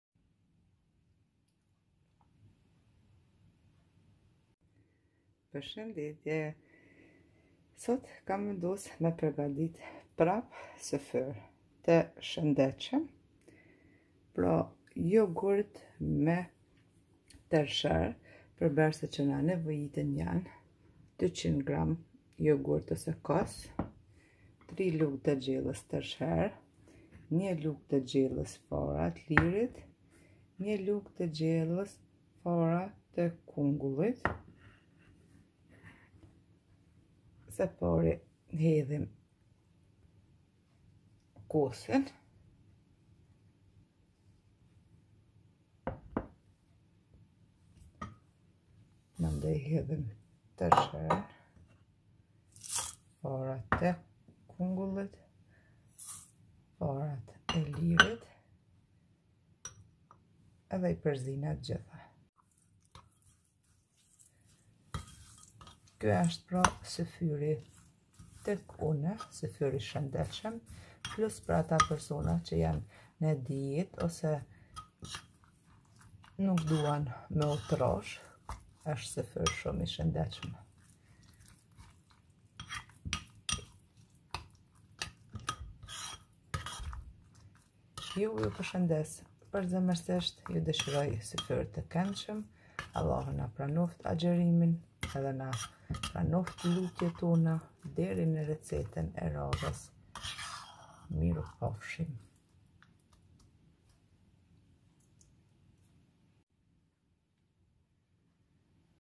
Syfyr i shendetshem dhe shum i mir per organizmin tonë,Gesunder Frühstück mit Haferflocken
Perbersit qe na nevojiten janë
200.gr Kos ose jogurt
3.lugë gjelle Tersher
1.lugë gjelle fara te kungullit
1.lugë gjelle fara te lirit

Sie  benötigen
200.gr Jogurt
3.EL Haferflocken
1.EL Leinsamen
1.EL Kürbiskerne